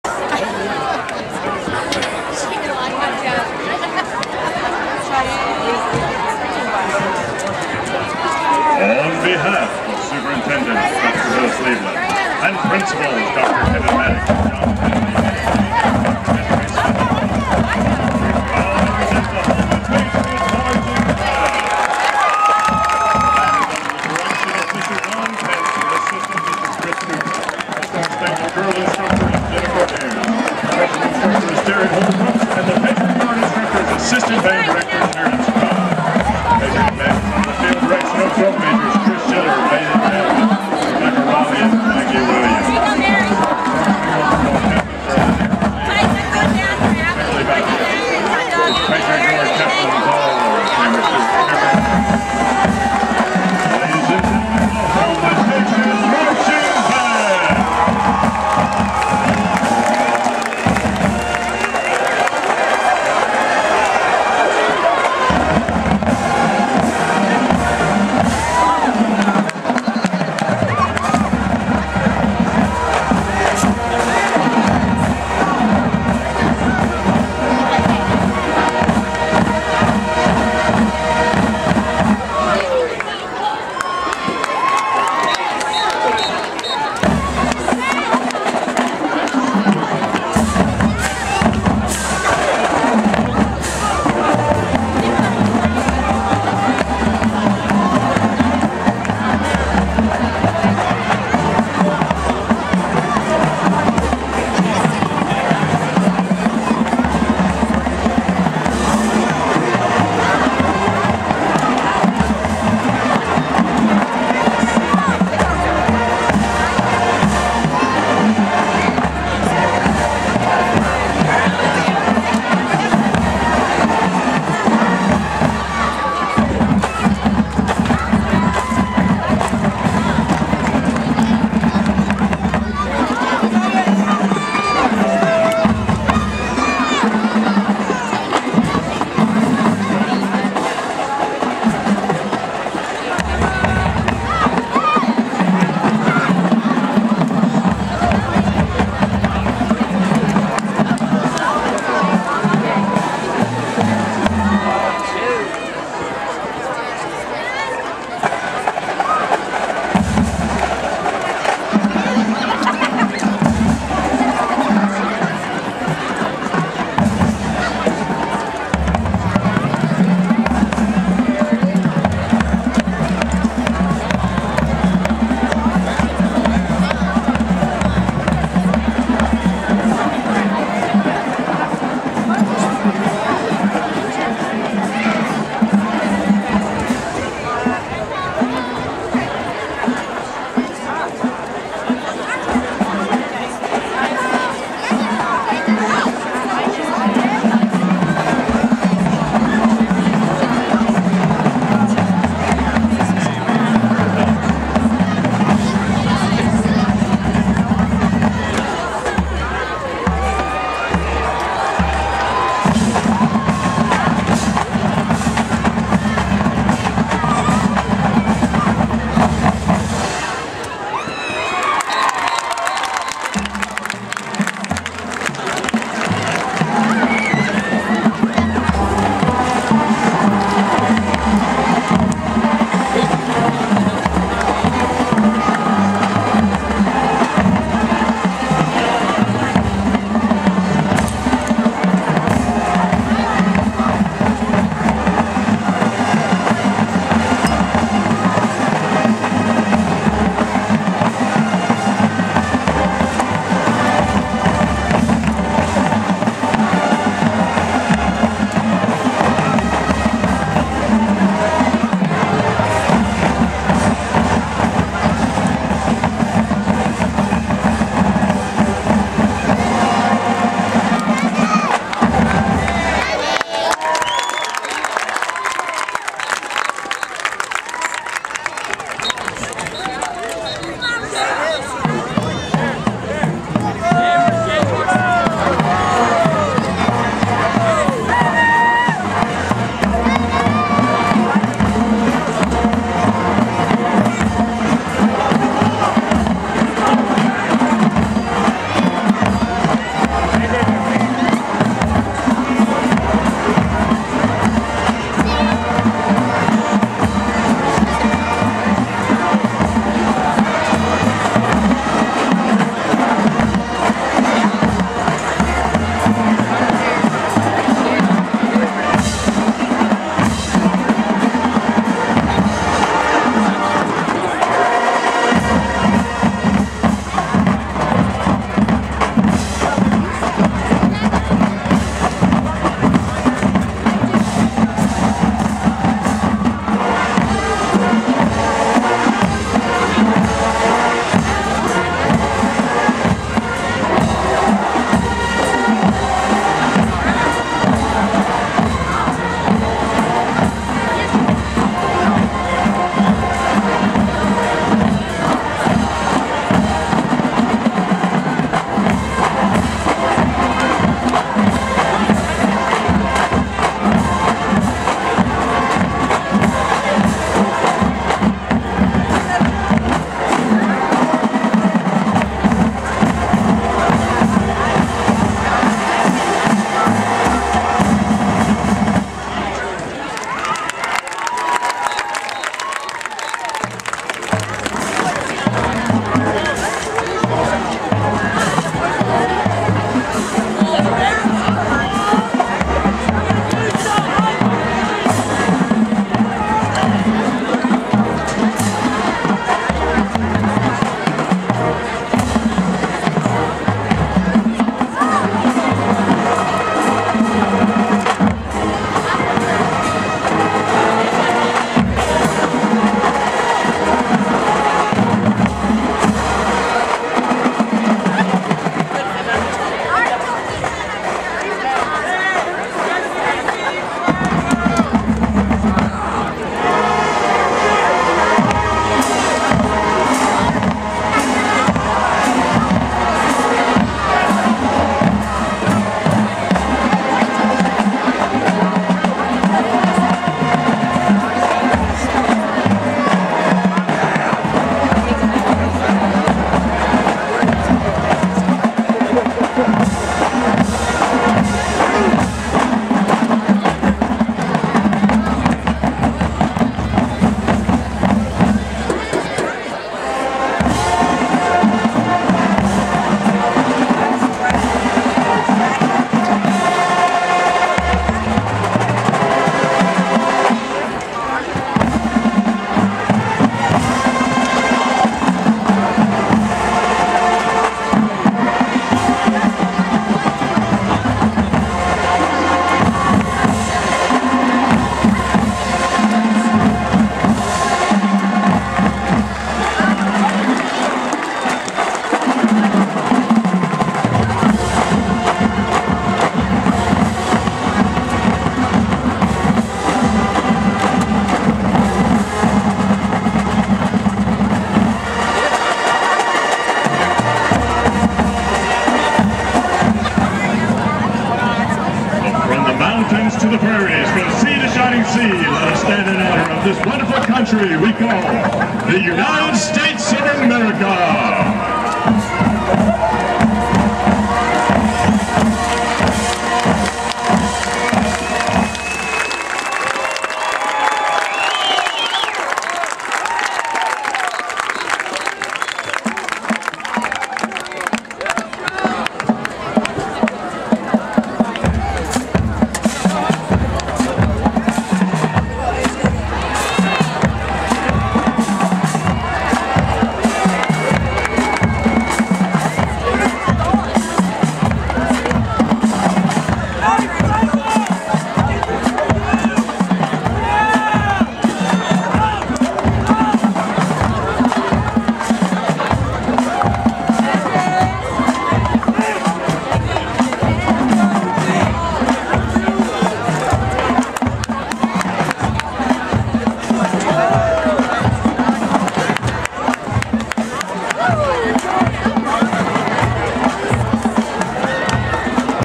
Homewood Patriots Band 2011 HHS @ Bessemer City
Homewood @ Bessemer City 26 Aug 2011.  Homewood High School Patriots Marching Band half-time show.